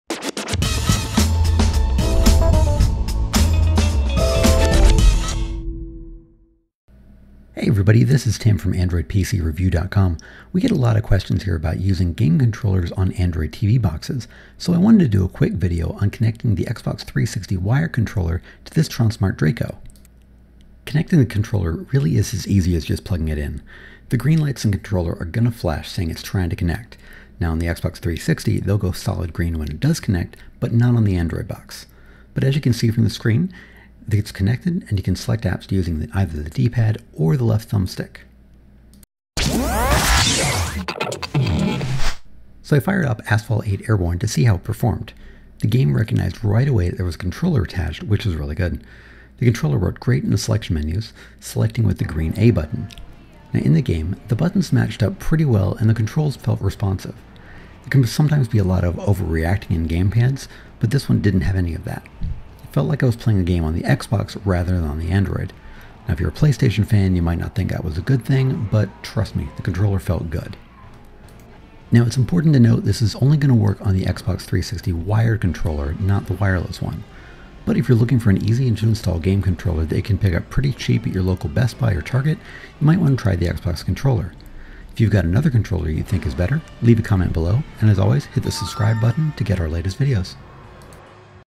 How to hook up an Xbox 360 Controller to an Android TV Box
How to hook up the Xbox 360 controller to an Android streaming TV box.